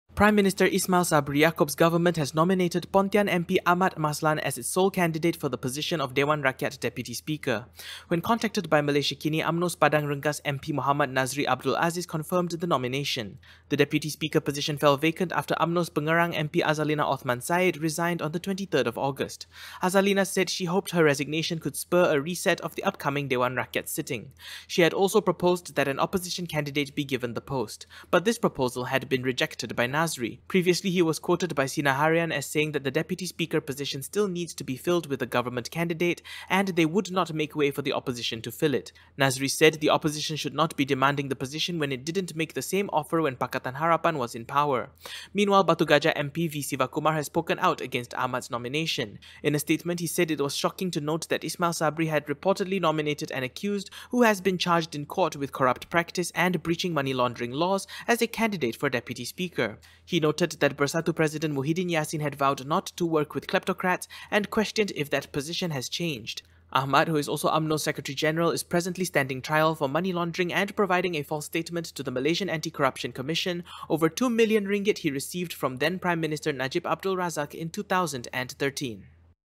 Ahmad Maslan nominated as Dewan Rakyat deputy speaker
Prime Minister Ismail Sabri Yaakob’s government has nominated Umno’s Pontian MP Ahmad Maslan as its sole candidate for the position of Dewan Rakyat deputy speaker.

Umno’s Padang Rengas MP Mohamed Nazri Abdul Aziz, when contacted, verified the nomination.

The deputy speaker position fell vacant after Pengerang MP Azalina Othman Said resigned on Aug 23.